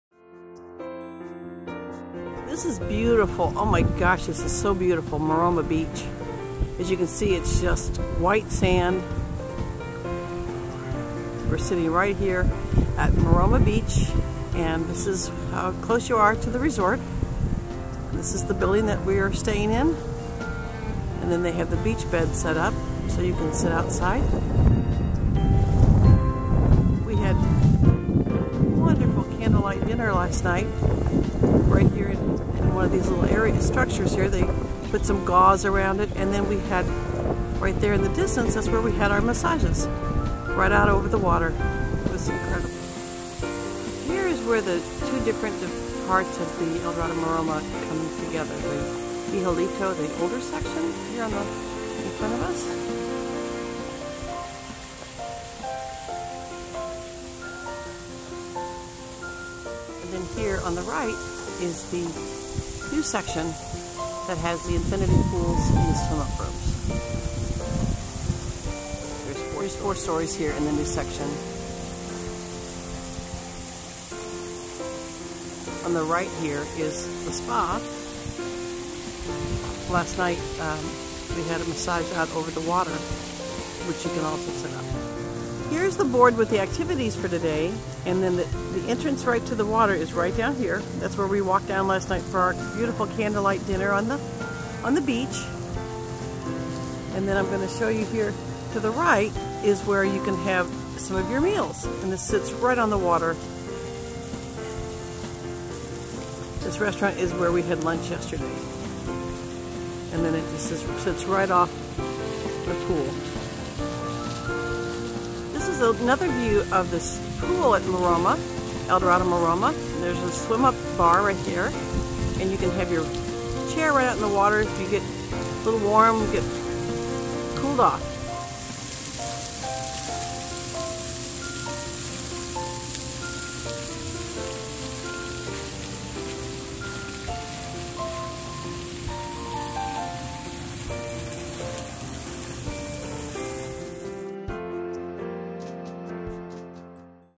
El Dorado Maroma
Cruise Holidays of Kansas City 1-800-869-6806
presents El Dorado Maroma, A Beachfront Resort, by Karisma
Located on one of Travel Channel's Top Ten Beaches in the World, El Dorado Maroma is an upscale, exclusive Caribbean resort for romantic couples and active vacationers alike.

On renowned Maroma beach in Mexico's Riviera Maya, El Dorado Maroma, a Beachfront Resort, by Karisma provides a secluded, enchanting setting for those looking to escape. Amidst the gorgeous Caribbean surroundings, this intimate resort with just 72 suites, is perfect for romantic getaways, destination weddings and sensational honeymoons.
When you've had your fill of sunbathing you'll discover a stimulating array of activities at El Dorado Maroma. Sport fishing, snorkeling, diving, kayaking and sailing are all offered within steps of the resort at Marina Maroma Paradise. And with weekly events, like our authentic fish market where guests select their own fish and have it grilled right on the beach, El Dorado Moroma offers plenty of opportunities for an unforgettable vacation.